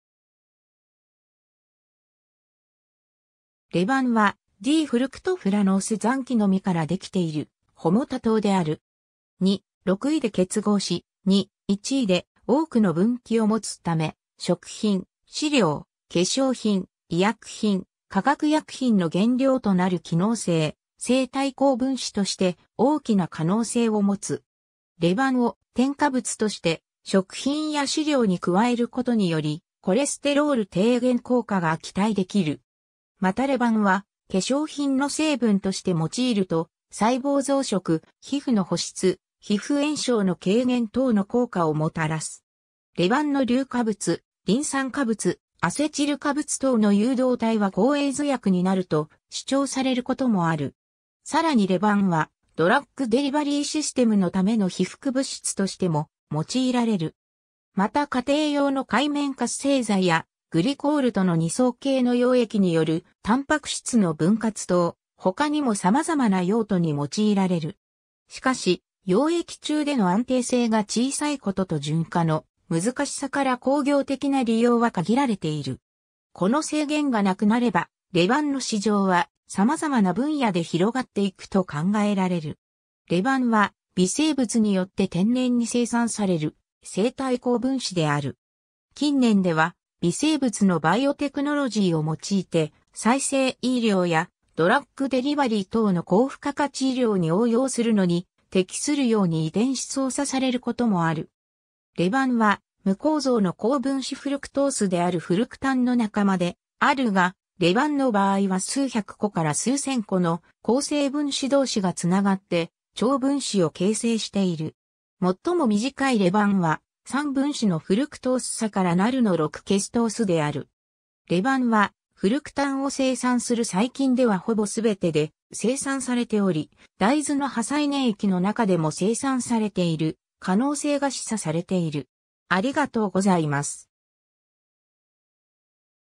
レバン
レバン(Levan)は、D-フルクトフラノース残基のみからできているホモ多糖である。
2，6位で結合し、2，1位で多くの分岐を持つため、食品、飼料、化粧品、医薬品、化学薬品の原料となる機能性生体高分子として大きな可能性を持つ。
レバンを添加物として食品や飼料に加えることにより、コレステロール低減効果が期待できる。
またレバンは化粧品の成分として用いると細胞増殖、皮膚の保湿、皮膚炎症の軽減等の効果をもたらす。
レバンの硫化物、リン酸化物、アセチル化物等の誘導体は抗エイズ薬になると主張されることもある。
さらにレバンはドラッグデリバリーシステムのための被覆物質としても用いられる。
また家庭用の界面活性剤や、グリコールとの二層系の溶液によるタンパク質の分割等、他にも様々な用途に用いられる。
しかし、溶液中での安定性が小さいことと純化の難しさから工業的な利用は限られている。
この制限がなくなれば、レバンの市場は様々な分野で広がっていくと考えられる。
 レバンは、微生物によって天然に生産される生体高分子である。
近年では、微生物のバイオテクノロジーを用いて、再生医療やドラッグデリバリー等の高付加価値医療に応用するのに適するように遺伝子操作されることもある。
 レバンは、無構造の高分子フルクトースであるフルクタンの仲間であるが、レバンの場合は数百個から数千個の構成分子同士がつながって、超分子を形成している。
 最も短いレバンは、3分子のフルクトース鎖からなるの6-ケストースである。
レバンは、フルクタンを生産する細菌ではほぼ全てで生産されており、大豆の破砕粘液の中でも生産されている可能性が示唆されている。